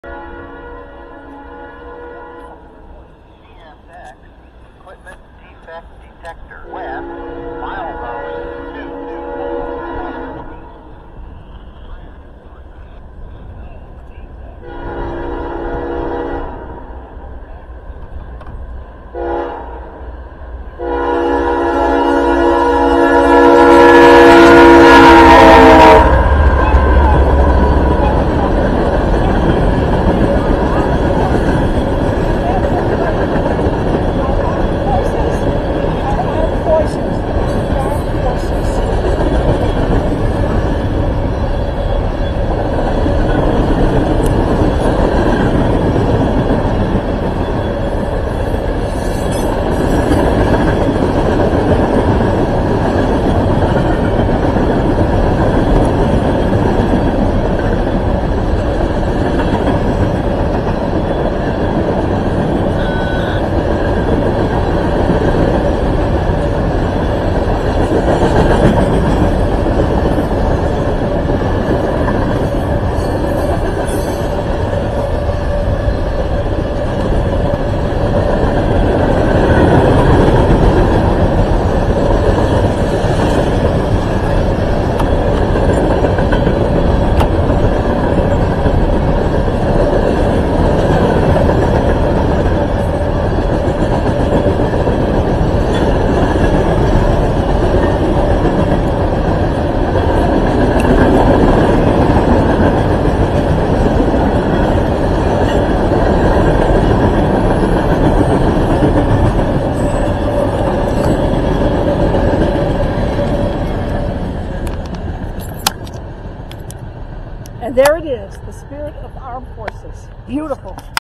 Coal Train lead by CSXT1776 Spirit of Our Armed Forces Locomotive.
Coal train lead by Spirit of Our Armed Forces locomotive going east on Garrett line in Suman, Indiana, 4-28-24. Train is shortened to 2 minutes at :38.